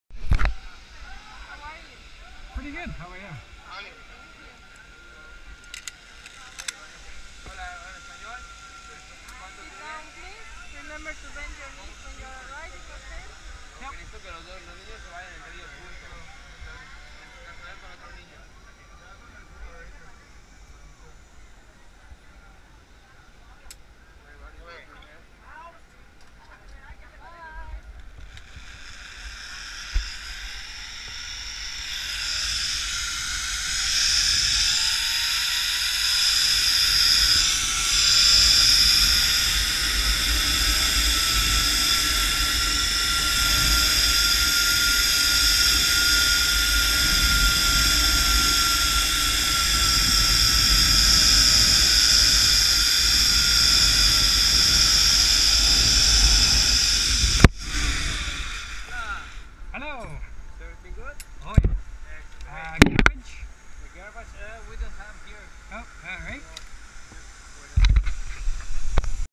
First Zip line
First zip line in Mexico at Xplor